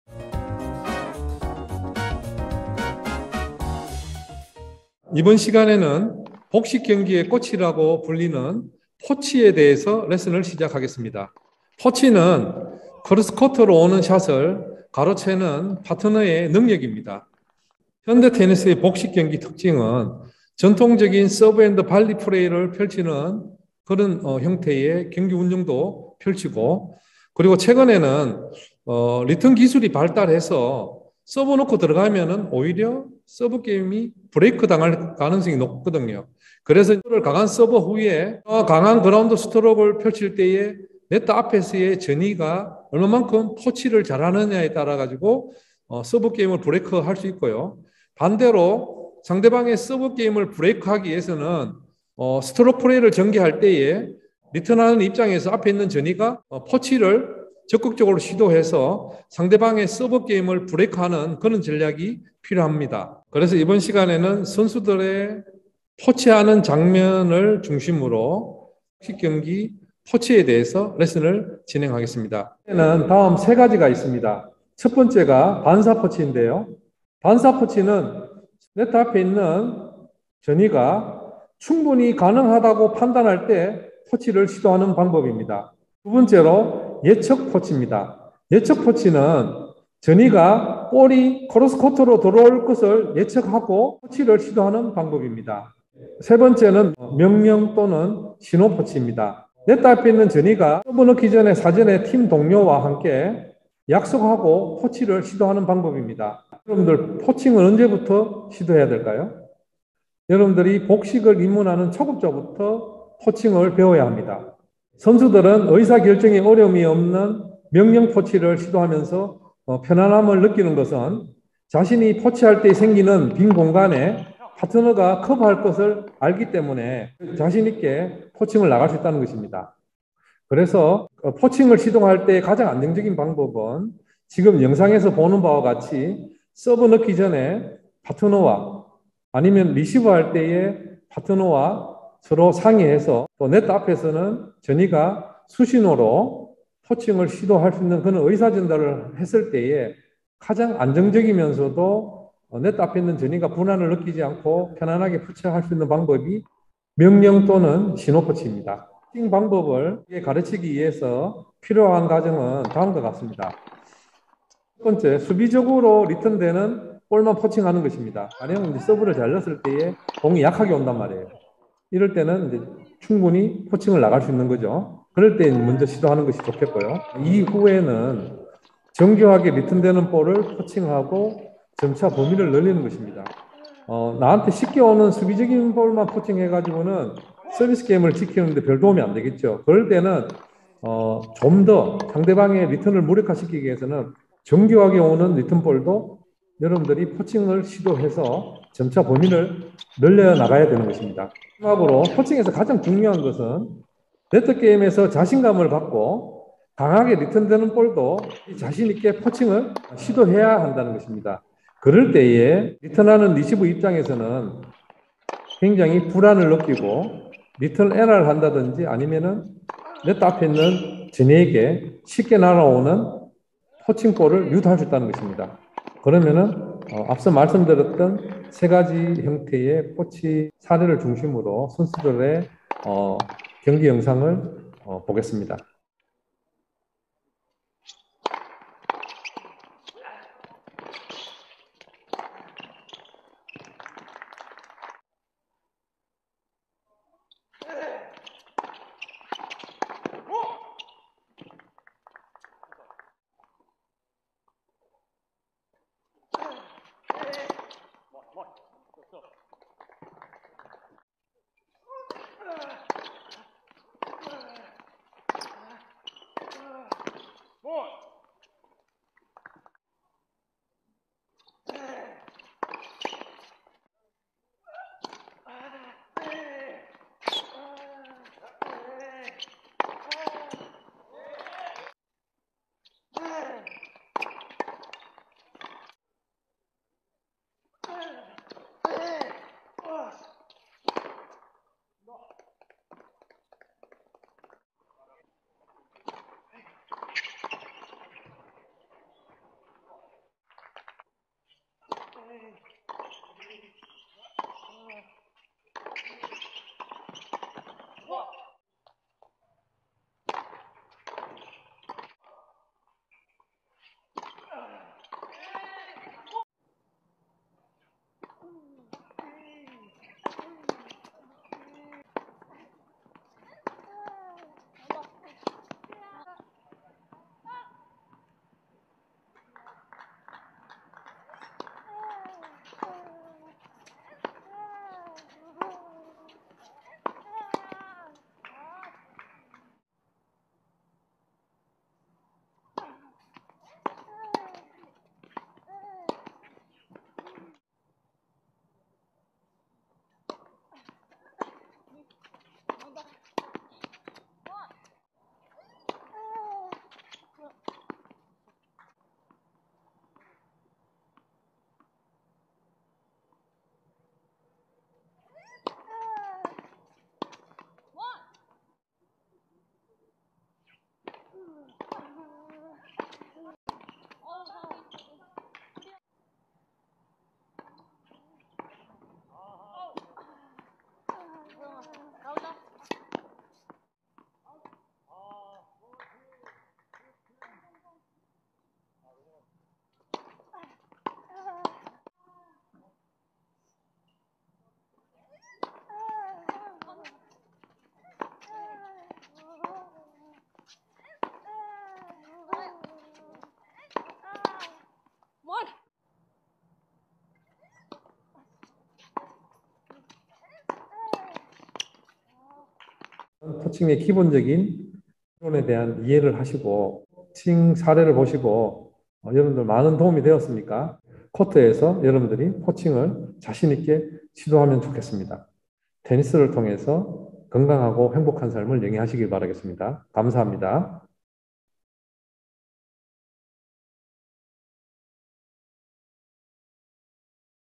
정진화테니스레슨-882회 포칭을 알면 당신은 테니스 복식 고수!
안녕하세요 정진화입니다. 테니스를 통해서 건강하고 행복한 삶을 영위하시기를 바랍니다. 감사힙니다.

정진화테니스아카데미
서울 은평구 응암로 310 파인빌딩 지하 1층. 블로그